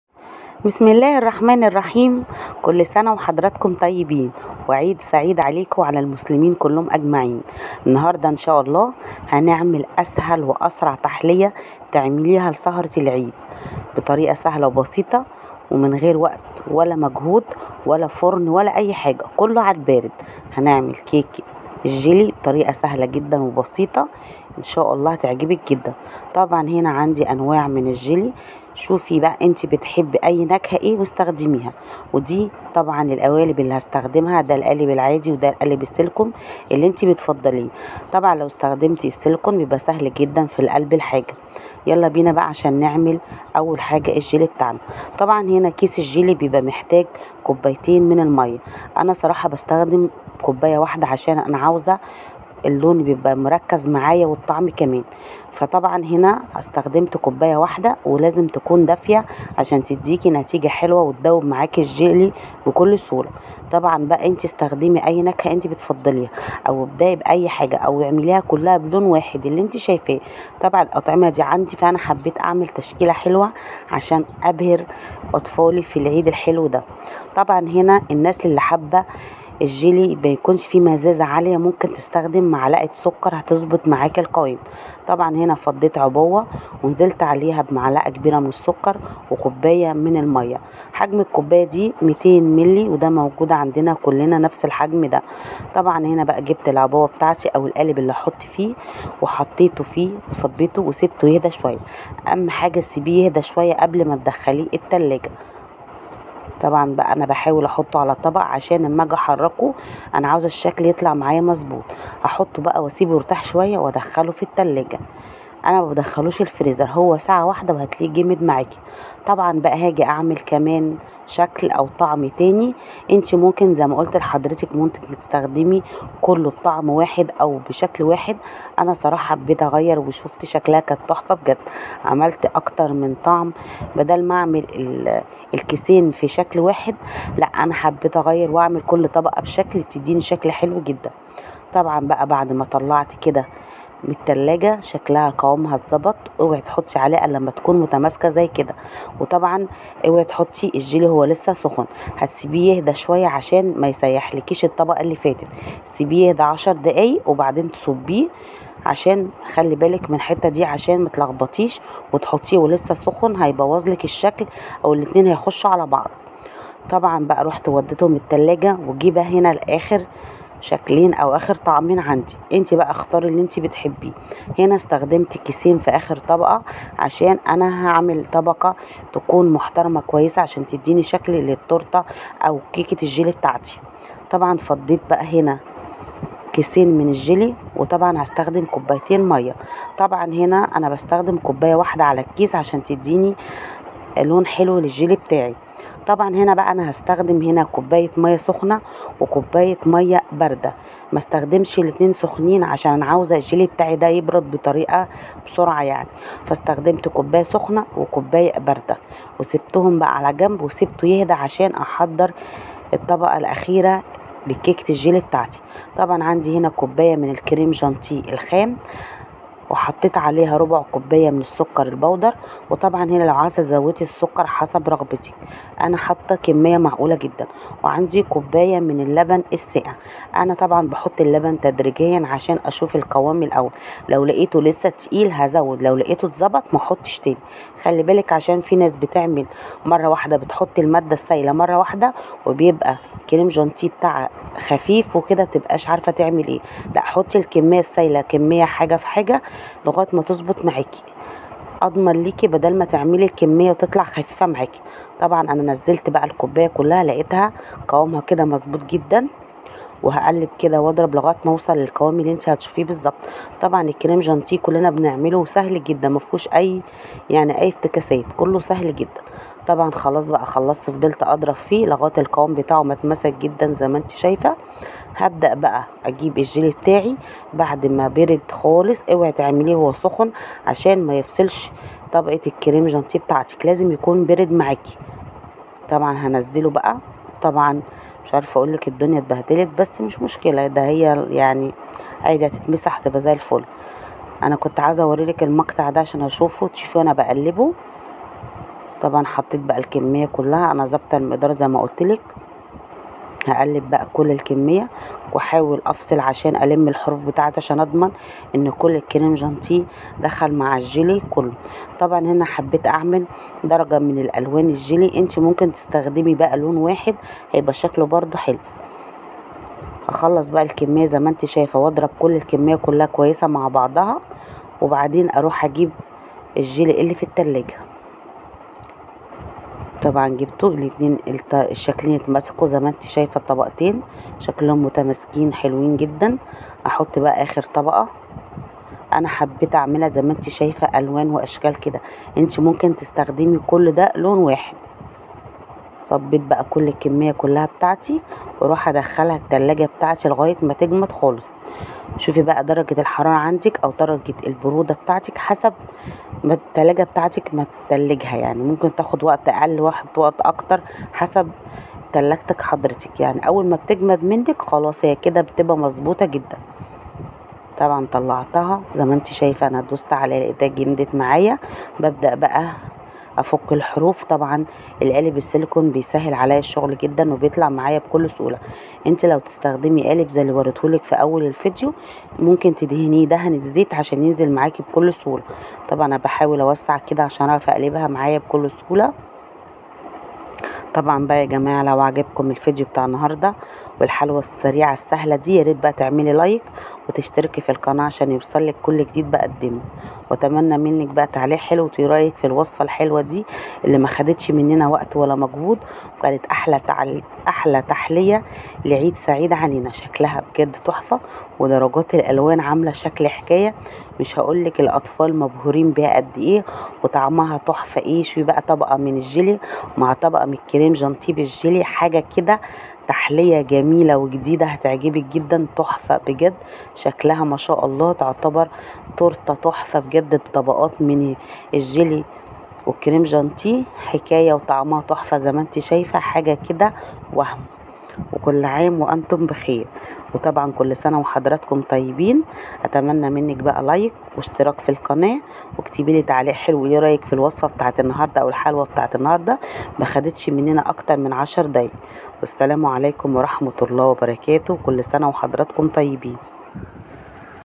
تحلية سهلة وسريعة بدون فرن وبدون بيض بدون مجهود تحليه باردة
تحلية سهلة وسريعة بدون فرن وبدون بيض لأحلى سهره لعيد الفطر